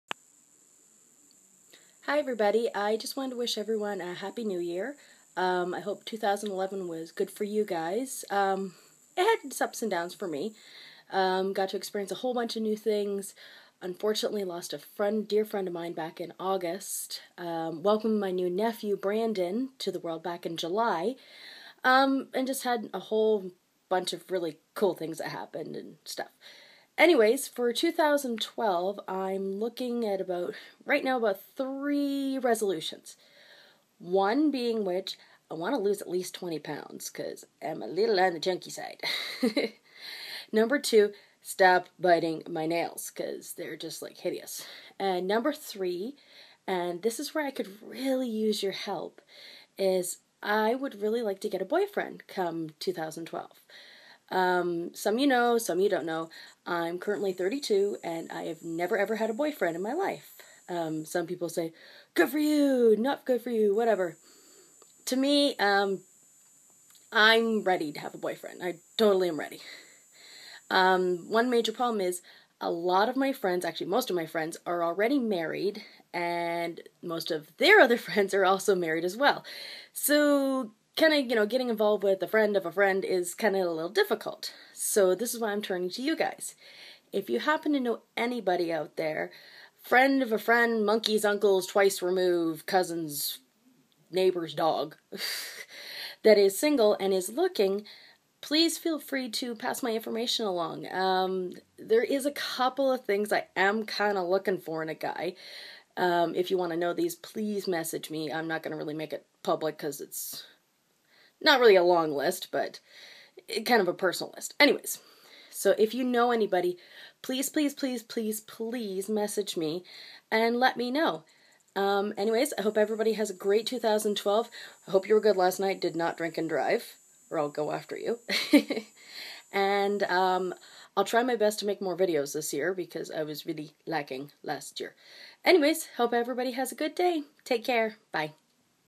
New Year Wishes and Resolutions
Hey all! Here's my first vid of 2012! I share my resolutions for this new year an I'm looking to YOU to help me with one of them.

I took this video with my iPhone  right before I headed out to a New Years party and didn't realize my finger was visible in the corner... Sorry about that.